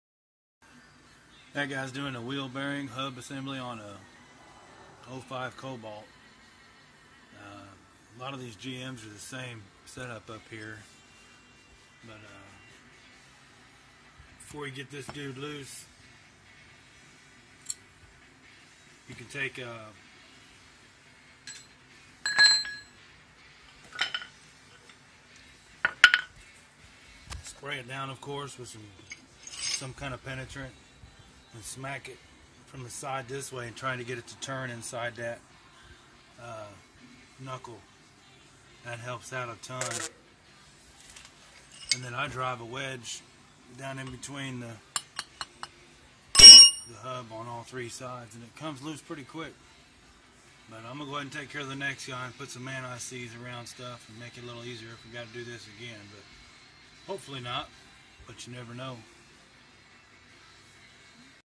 Cobalt Hub Swap Tip
Safety First Jack stand with a jack ! lil tip if ya need to replace hub/bearing assembly on small-mid size late model GM